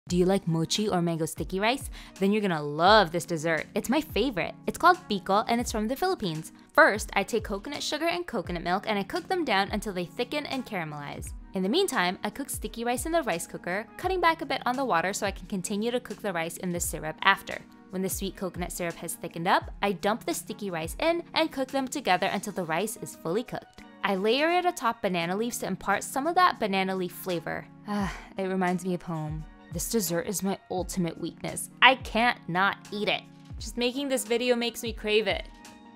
Caramelized Coconut Sticky Rice is my FAVORITE Filipino dessert
There are a whole variety of Filipino rice-based desserts called "kakanin". They're ALL delicious! I have a few full-video recipes on the channel in a playlist 😊